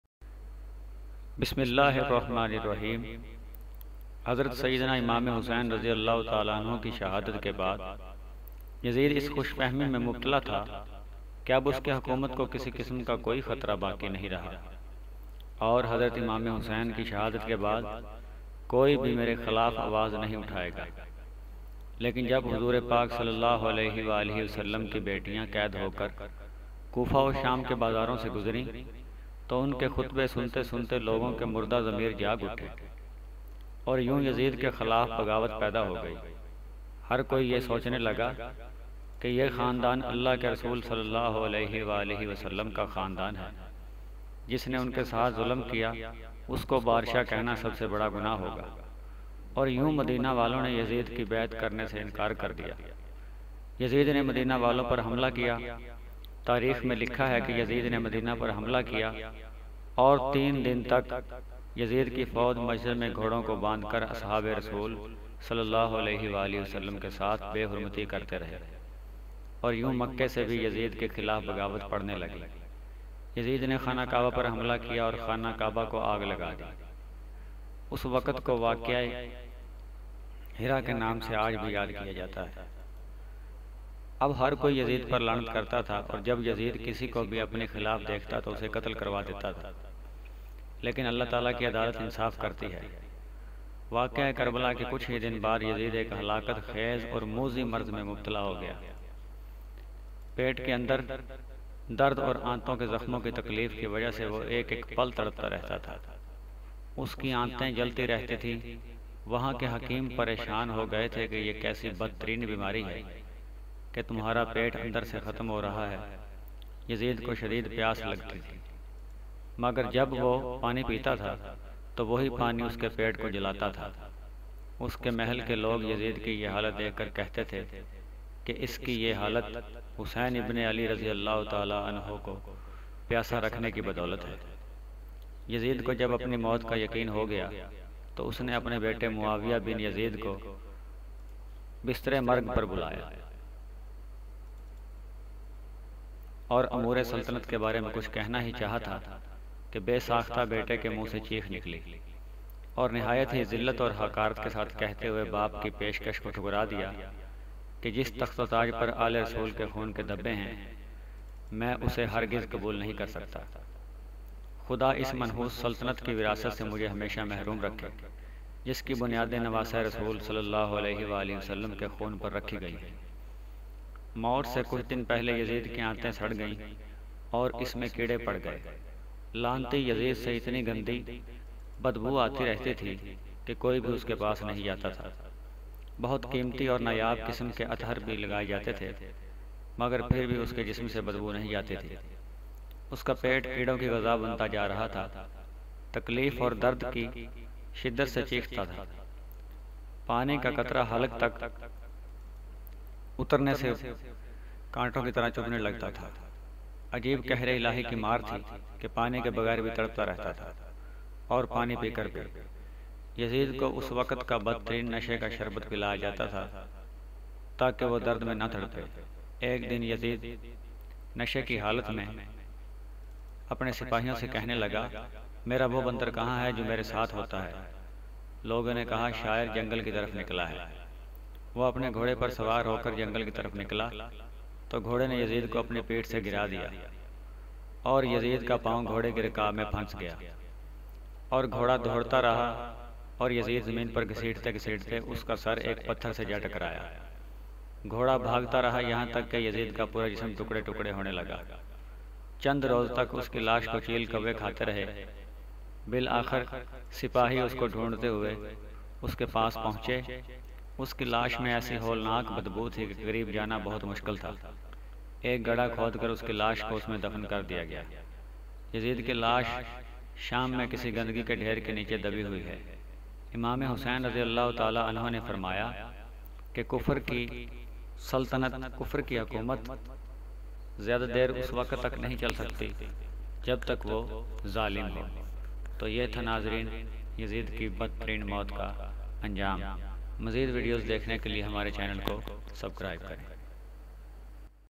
Yazid Ka Dard Naak Anjaam | Yazid Ki Death |Death Of Yazeed
yazeed ka anjaam is video me ap dekh saken ge k yazeed kese mara tha yazid ka ibrat naak anjaam